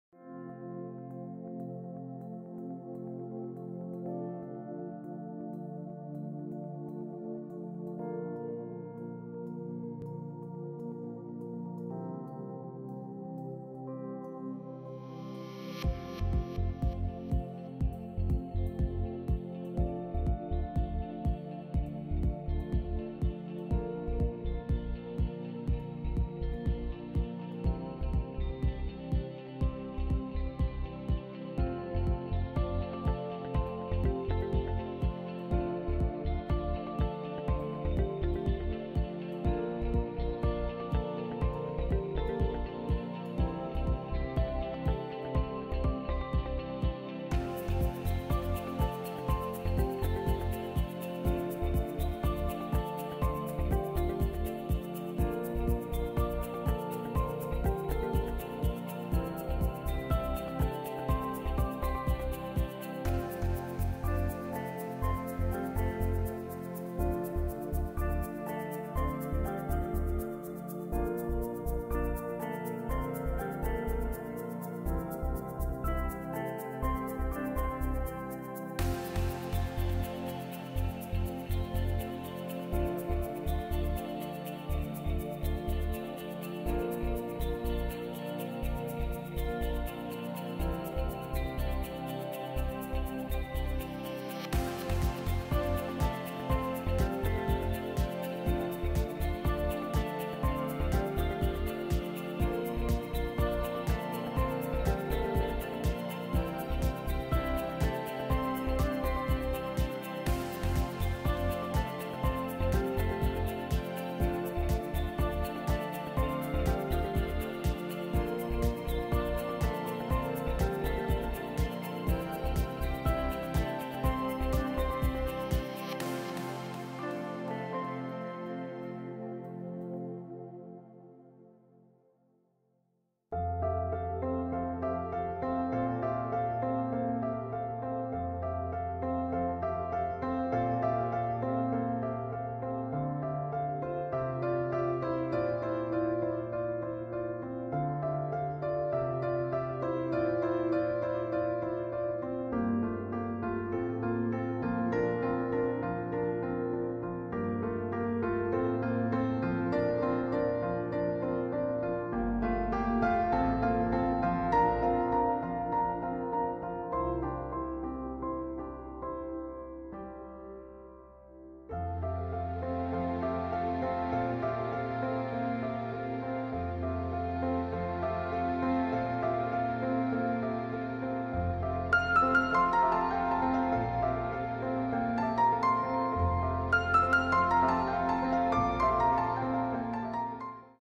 Acrylic on Canvas_Pink Lotus#acrylic
Trying to portray a realistic image of pink lotus and leaves with water drops.